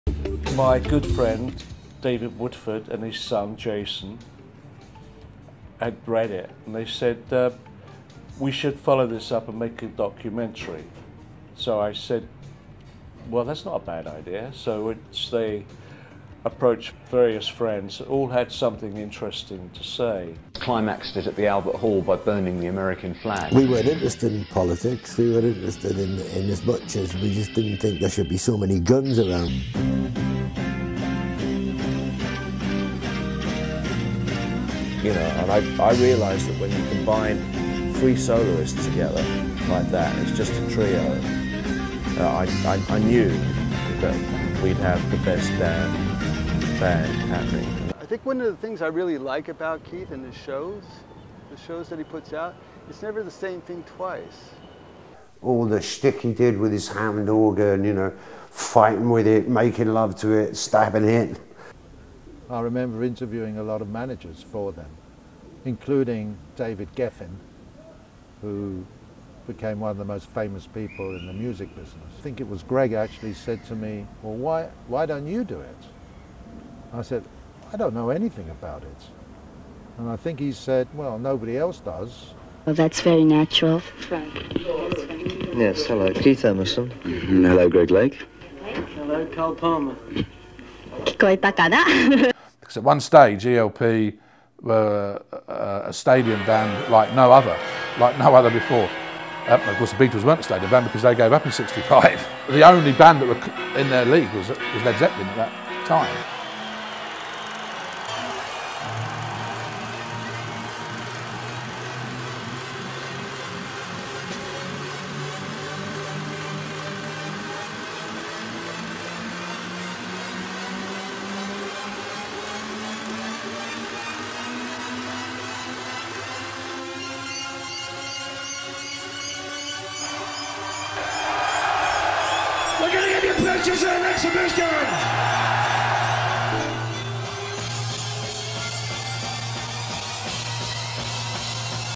Keith Emerson Doc Indiegogo 2016 Campaign - Video #1
Keith Emerson's (Emerson, Lake and Palmer) life story was first published in 2003, and we have been working on this adaptation and update since 2007. Thank you to all our supporters thus far, who have shared our dream and turning it to reality...